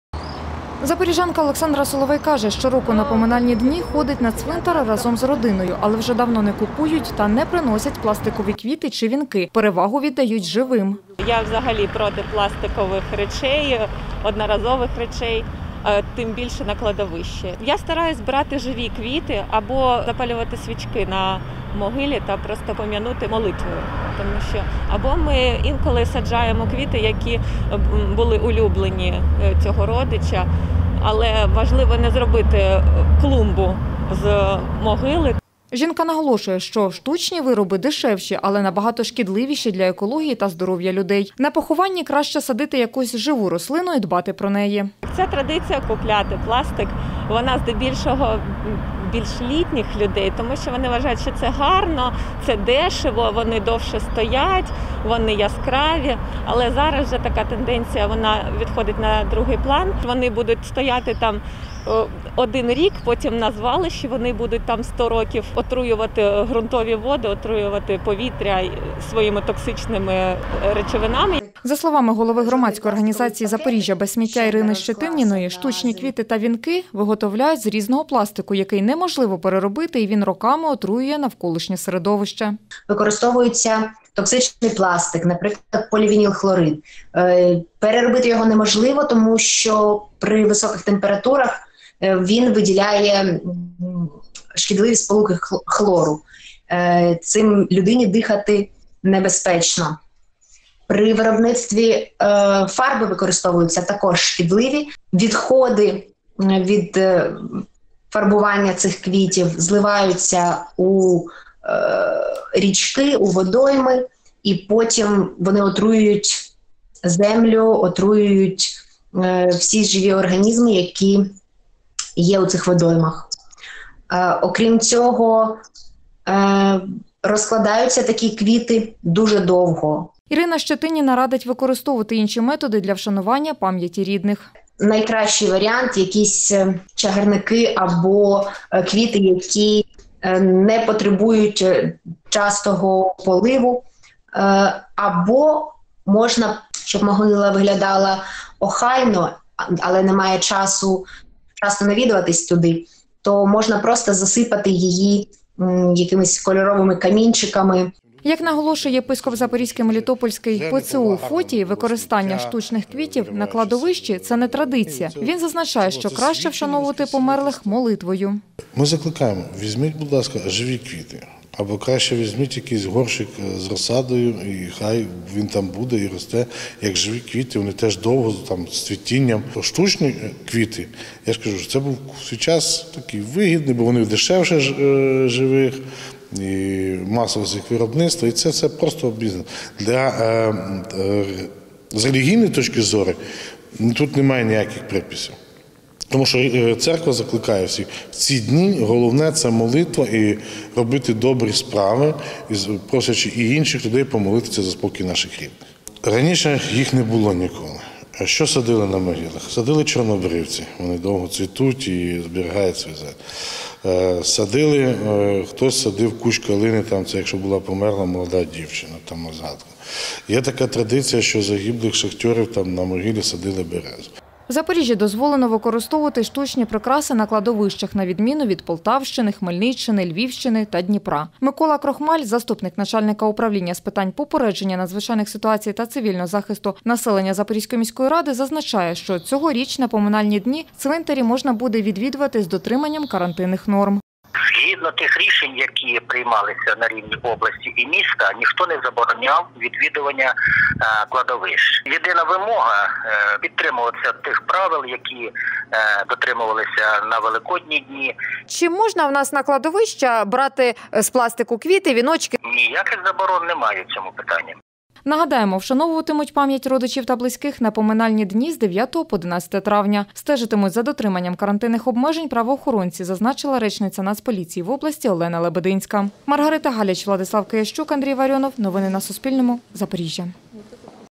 Альтернатива штучним квітам для вшанування померлих | Новини
Штучні пластикові вінки та квіти на поминальні дні: традиція чи економія? Чи шкідливі вони для екології та здоров'я людей, та яка є альтернатива таким прикрасам та способам вшанування пам'яті рідних? Відповіді на ці запитання шукали журналісти Суспільного.